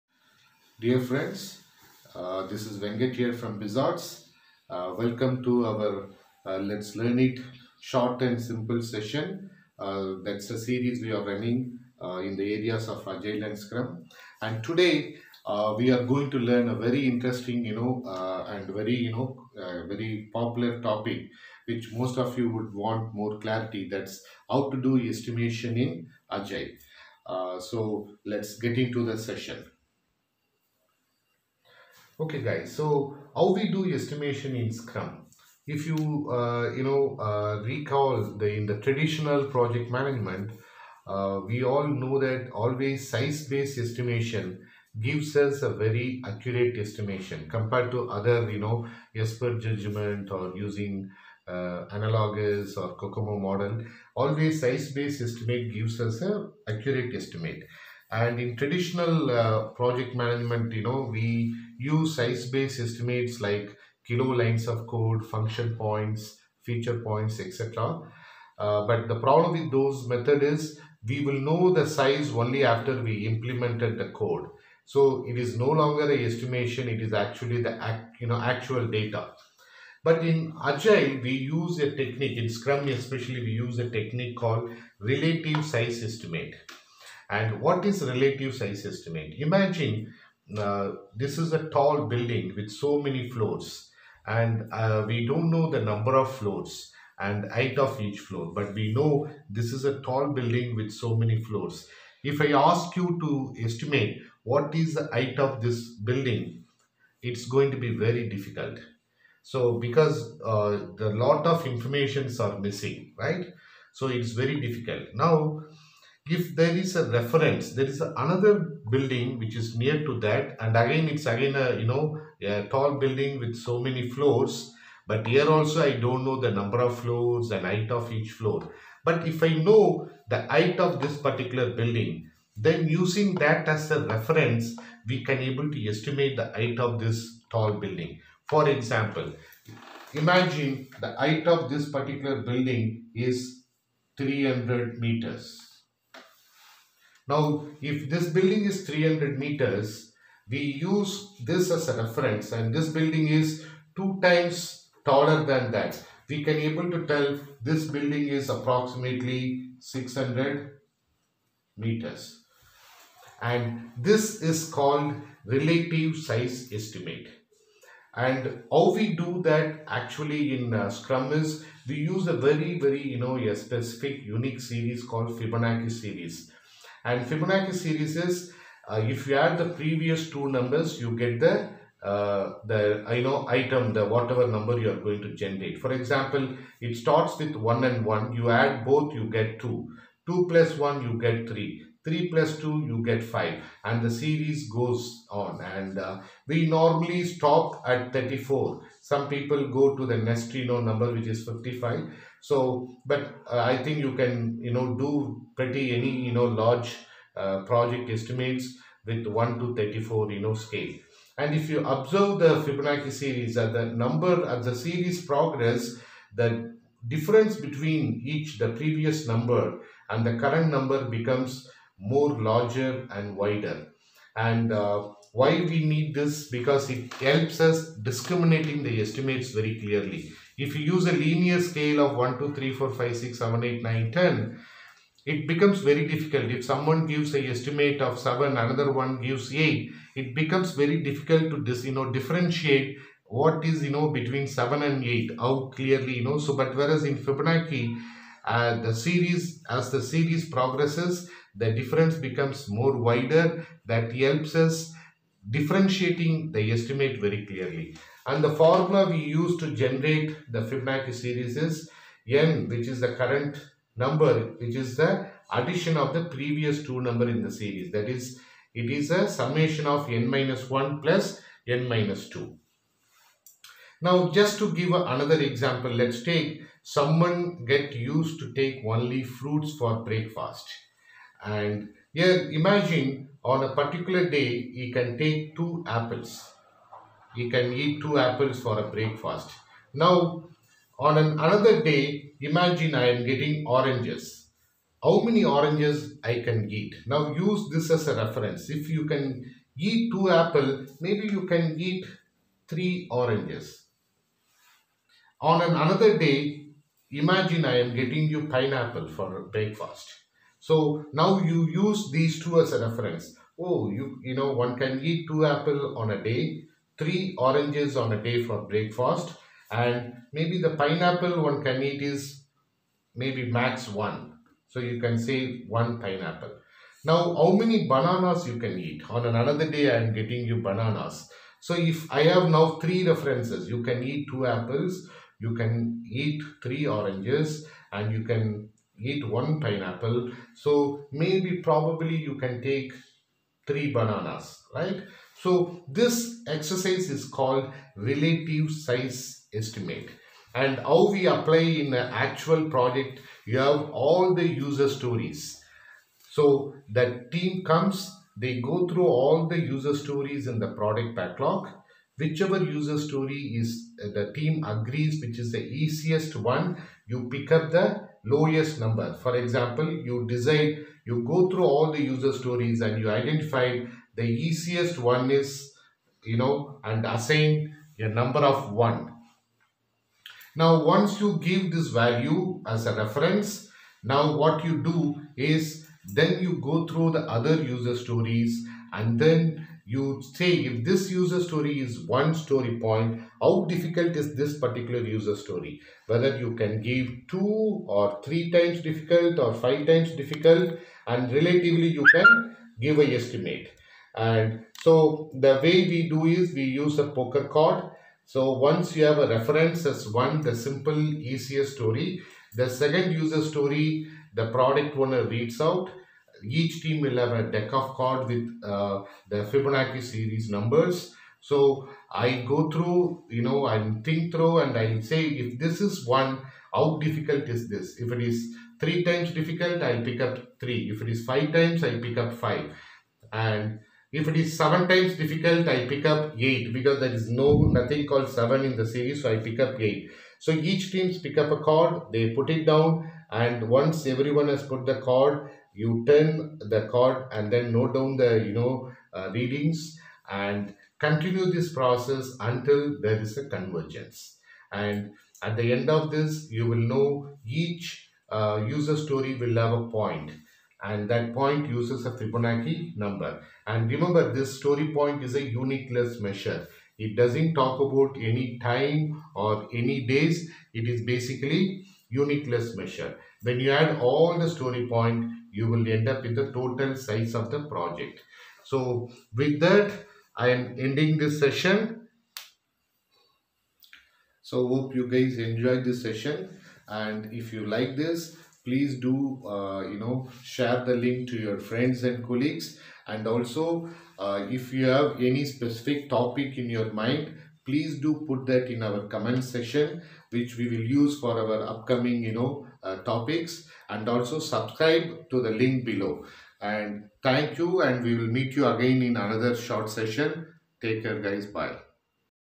Relative Size/ Story Point / Agile Estimation in 7 minutes - LISS #4
Lets Learn It Short and Simple( LISS) series on Agile scrum by Venkat